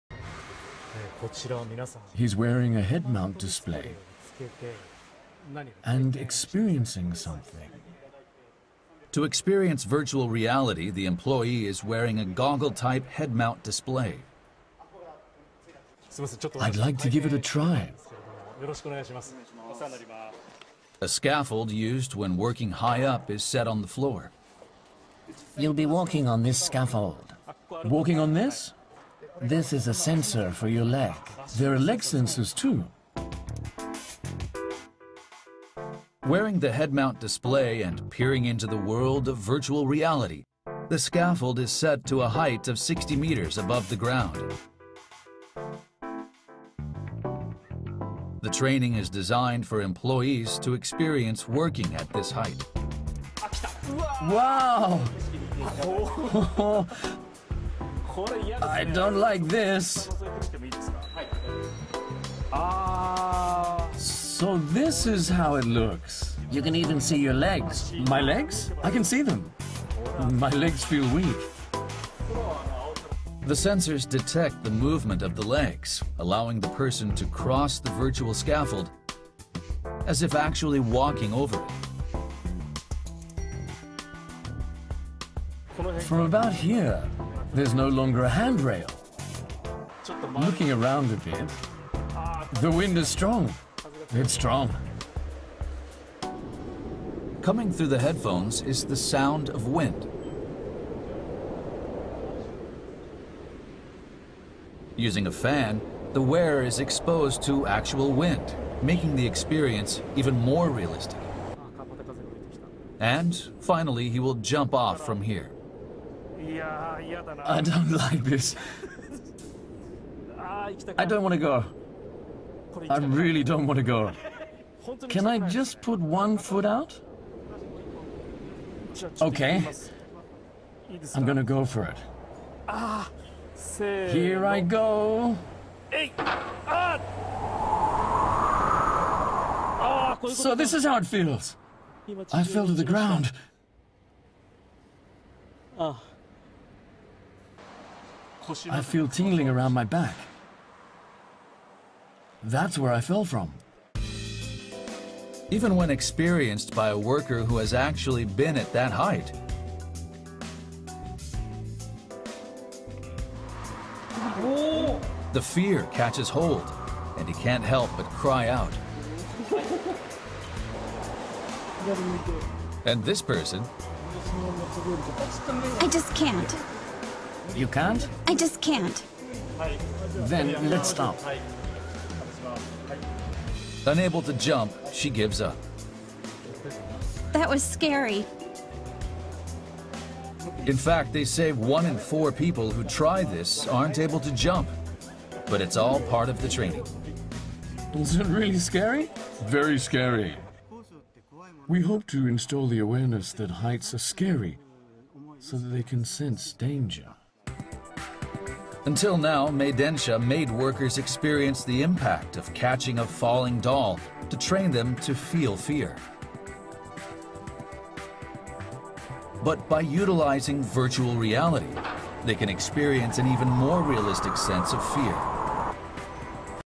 Huấn luyện an toàn trên cao bằng Công nghệ Thực Tế Ảo
Việc huấn luyện an toàn trên cao rất quan trọng, nếu chúng ta không được huấn luyện kỹ lưỡng, thực hành thực tế thì ra ngoài làm công việc rất nguy hiểm.